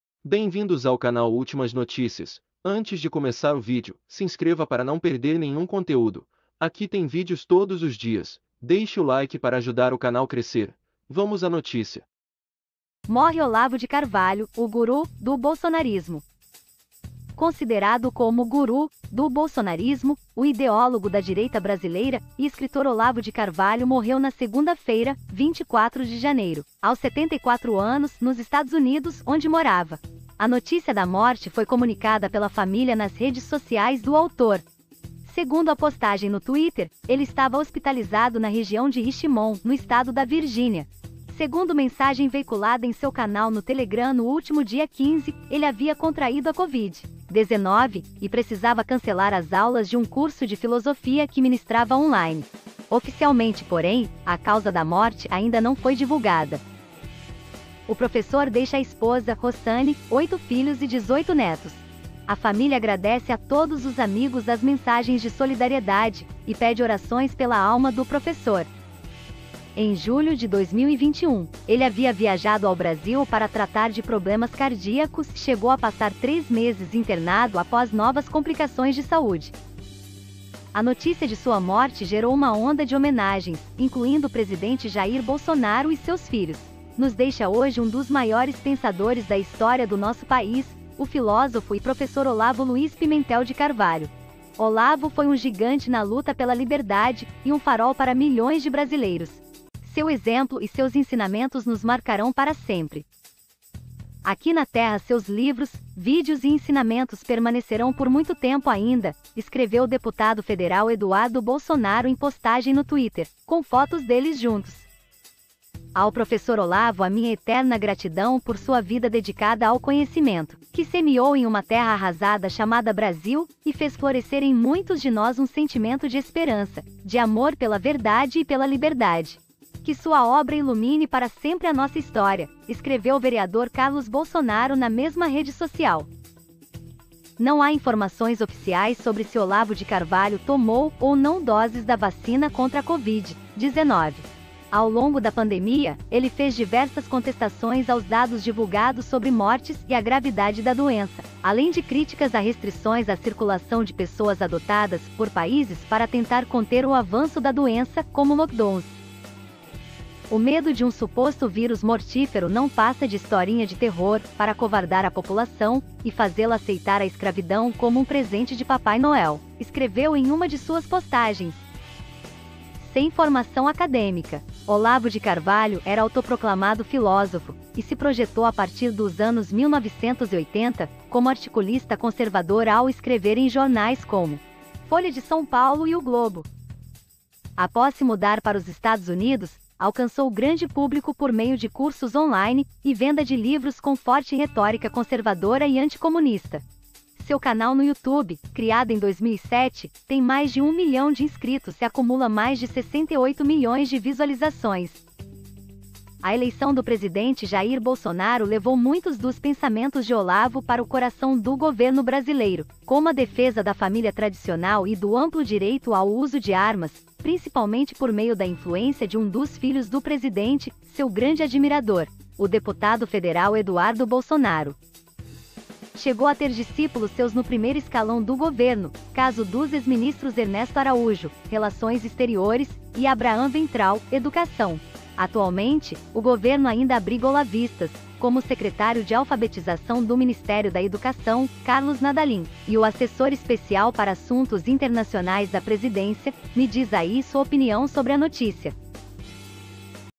ÚLTIMAS NOTÍCIAS | Olavo de Carvalho o "GURU" do BOLSONARO
Morre Olavo de Carvalho, o 'guru' do bolsonarismo

Considerado como "guru" do bolsonarismo, o ideólogo da direita brasileira e escritor Olavo de Carvalho morreu na segunda-feira, 24 de janeiro, aos 74 anos, nos Estados Unidos, onde morava. Segundo a postagem no Twitter, ele estava hospitalizado na região de Richmond, no estado da Virgínia. Segundo mensagem veiculada em seu canal no Telegram no último dia 15, ele havia contraído a Covid-19 e precisava cancelar as aulas de um curso de filosofia que ministrava online.  Oficialmente, porém, a causa da morte ainda não foi divulgada.

"O professor deixa a esposa, Roxane, oito filhos e 18 netos. A família agradece a todos os amigos as mensagens de solidariedade e pede orações pela alma do professor",

Em julho de 2021, ele havia viajado ao Brasil para tratar de problemas cardíacos e chegou a passar três meses internado após novas complicações de saúde.

A notícia de sua morte gerou uma onda de homenagens, incluindo o presidente Jair Bolsonaro e seus filhos: "Nos deixa hoje um dos maiores pensadores da história do nosso país, o filósofo e professor Olavo Luiz Pimentel de Carvalho. Olavo foi um gigante na luta pela liberdade e um farol para milhões de brasileiros. Seu exemplo e seus ensinamentos nos marcarão para sempre",

"Aqui na Terra seus livros, vídeos e ensinamentos permanecerão por muito tempo ainda", escreveu o deputado federal Eduardo Bolsonaro em postagem no Twitter, com fotos deles juntos.6

"Ao Professor Olavo a minha eterna gratidão por sua vida dedicada ao conhecimento, que semeou em uma terra arrasada chamada Brasil e fez florescer em muitos de nós um sentimento de esperança, de amor pela verdade e pela liberdade. Que sua obra ilumine para sempre a nossa história!", escreveu o vereador Carlos Bolsonaro na mesma rede social.

Não há informações oficiais sobre se Olavo de Carvalho tomou ou não doses da vacina contra a Covid-19. Ao longo da pandemia, ele fez diversas contestações aos dados divulgados sobre mortes e a gravidade da doença, além de críticas a restrições à circulação de pessoas adotadas por países para tentar conter o avanço da doença, como lockdowns.

"O medo de um suposto vírus mortífero não passa de historinha de terror para acovardar a população e fazê-la aceitar a escravidão como um presente de Papai Noel", escreveu em uma de suas postagens.

Sem formação acadêmica, Olavo de Carvalho era autoproclamado filósofo e se projetou a partir dos anos 1980 como articulista conservador ao escrever em jornais como Folha de S.Paulo e O Globo.

Após se mudar para os Estados Unidos, alcançou grande público por meio de cursos online e venda de livros com forte retórica conservadora e anticomunista. Seu canal no YouTube, criado em 2007, tem mais de um milhão de inscritos e acumula mais de 68 milhões de visualizações.

A eleição do presidente Jair Bolsonaro levou muitos dos pensamentos de Olavo para o coração do governo brasileiro, como a defesa da família tradicional e do amplo direito ao uso de armas, principalmente por meio da influência de um dos filhos do presidente, seu grande admirador, o deputado federal Eduardo Bolsonaro.

Chegou a ter discípulos seus no primeiro escalão do governo, caso dos ex-ministros Ernesto Araújo (Relações Exteriores) e Abraham Weintraub (Educação). Atualmente, o governo ainda abriga olavistas, como o secretário de Alfabetização do Ministério da Educação, Carlos Nadalim, e o Assessor Especial para Assuntos Internacionais da presidência,

Me diz aí  sua opinião sobre a notícia.